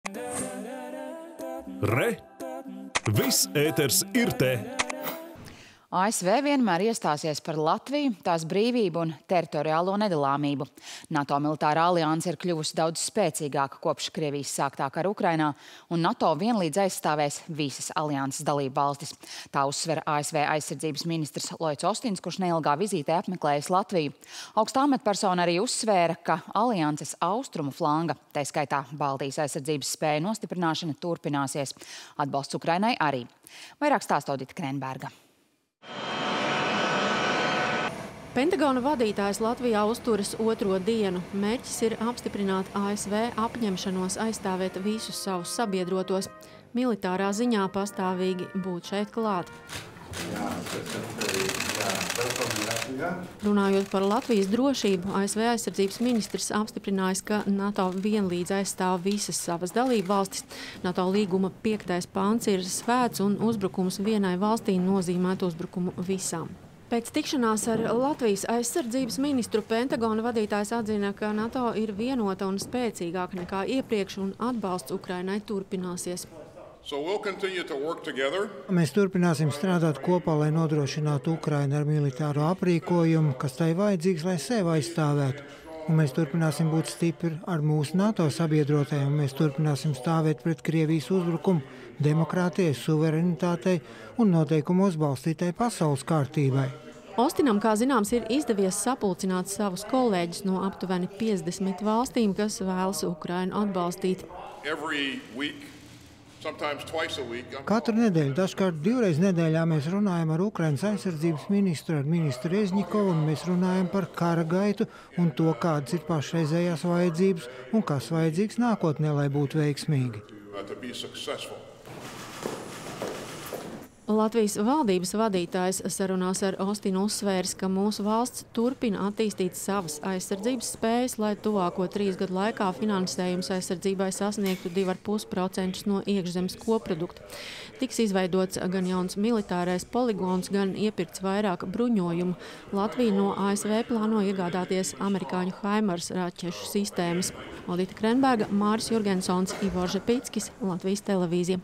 ASV aizsardzības ministra L.Ostina vizīte Latvijā
ASV vienmēr iestāsies par Latviju, tās brīvību un teritoriālo nedalāmību. NATO militārā alianse ir kļuvusi daudz spēcīgāka kopš Krievijas sāktā kara Ukrainā. NATO vienlīdz aizstāvēs visus savus locekļus. Un alianses austrumu flanga, tai skaitā Baltijas aizsardzības spēju nostiprināšana turpināsies. Atbalsts Ukrainai – arī. Tie ir galvenie vēstījumi no ASV aizsardzības ministra Loida Ostina, kurš apmeklējis vizītē Latviju. Vairāk stāsta Odita Krenberga.